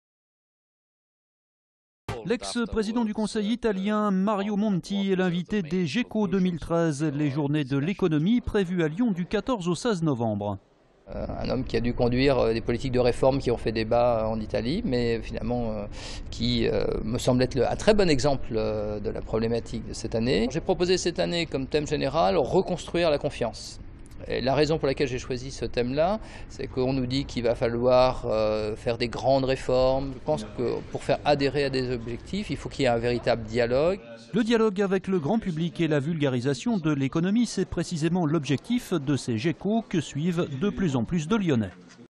Mario Monti invité des Jéco
Sites webs/blogs, copiez cette vidéo avec le bouton "Intégrer" !
 L'ex-président du conseil italien Mario Monti est l'invité des journées de l'économie.